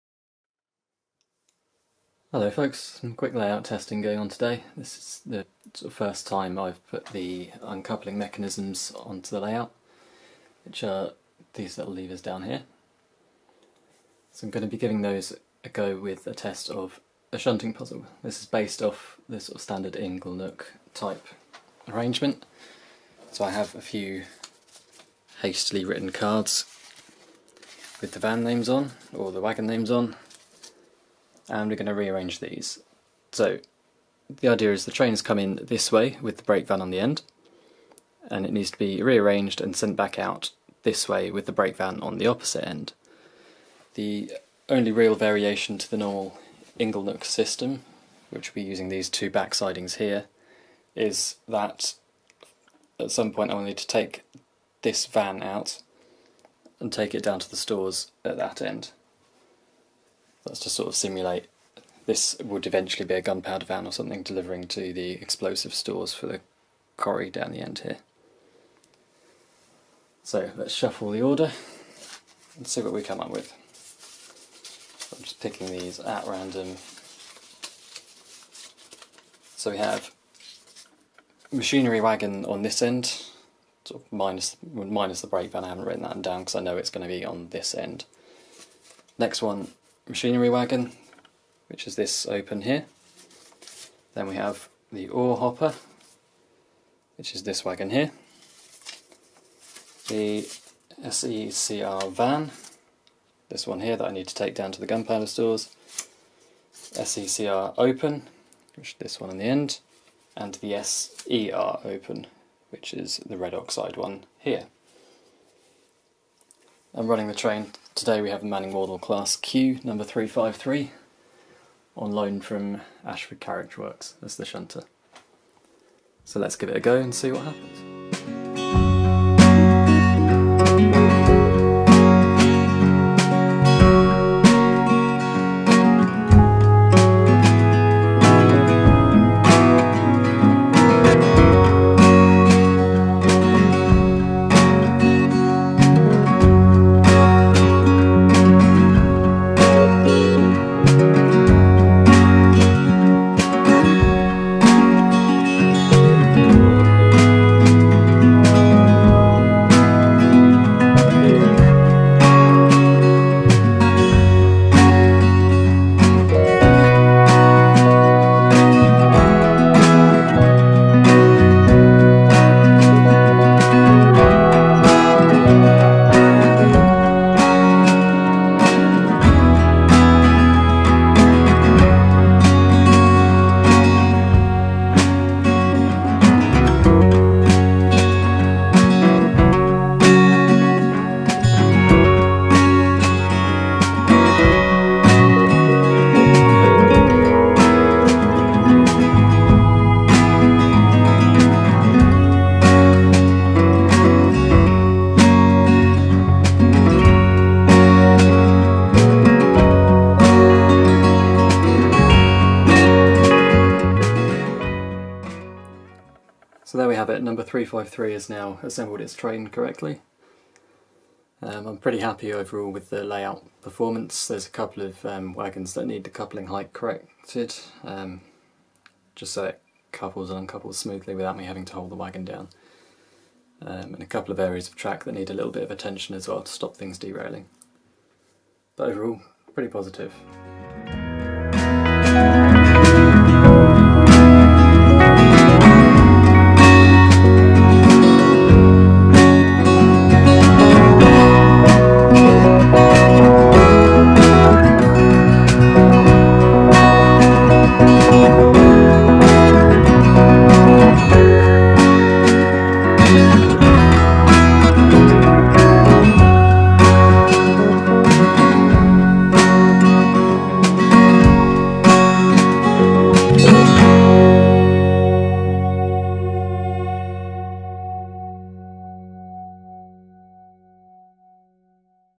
Testing the Shunting Puzzle: first test with uncoupling system!
This was the layout's first real test as a mostly complete system - remote uncoupling sections have been added to make the shunting puzzle hands-free. Also trying out the variations on the standard Inglenook puzzle for the first time.